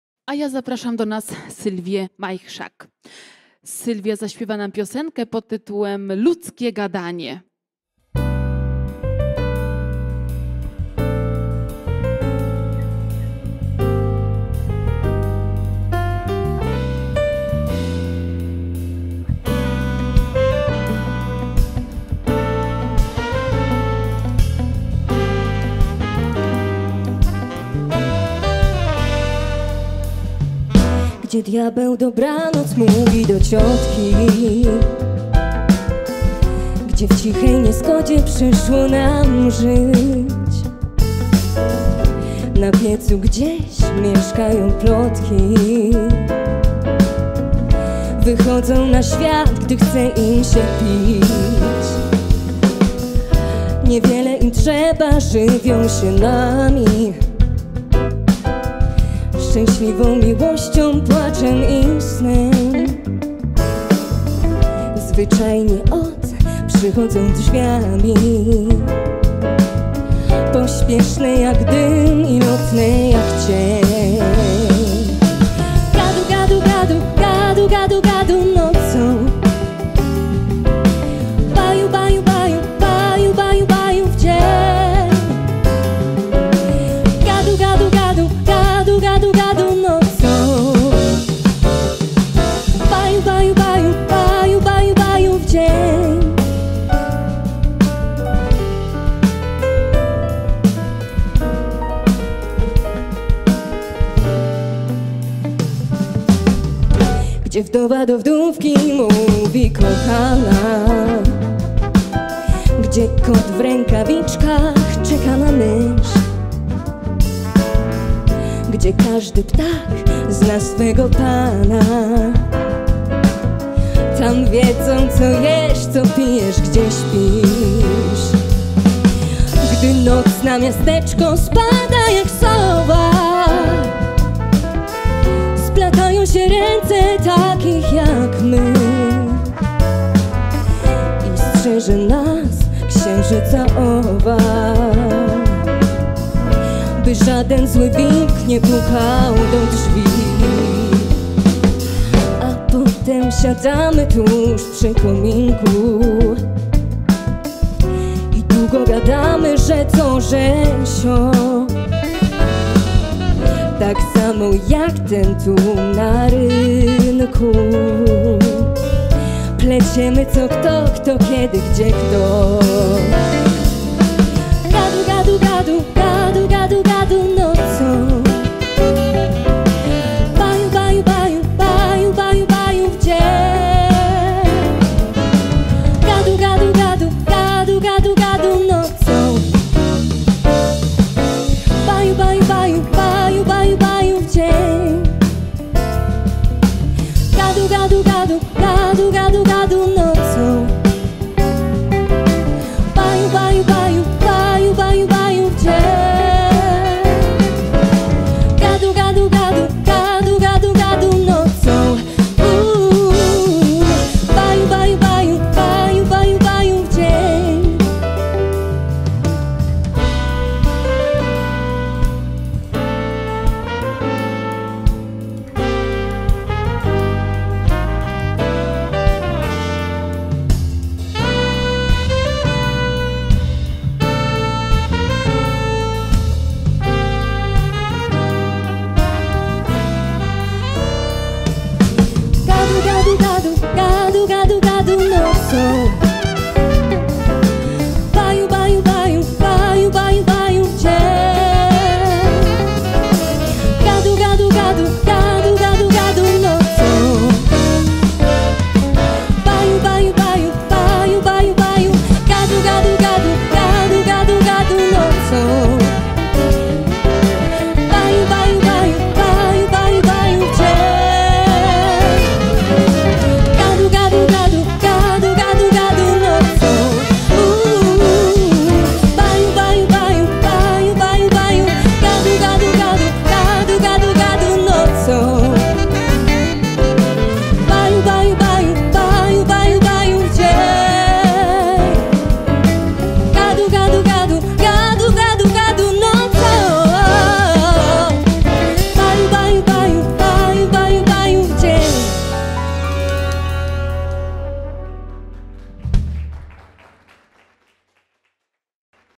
OFPHH 2018 - Sylwia Majchrzak
Nagranie z 29. Koncertu Konkursowego Ogólnopolskiego Festiwalu Piosenki im. Henryka Hampla.

OFPHH Band:
perkusja | Szymon Bisaga
gitara basowa | Bogumił Eksner
piano | Miłosz Bazarnik
gitary: elektryczna, akustyczna | Jarosław Szulc
saksofon, klarnet | Jarosław Kaczmarczyk
trąbka | Zbigniew Szwajdych

Realizacja dźwięku live:
Karol Mankiewicz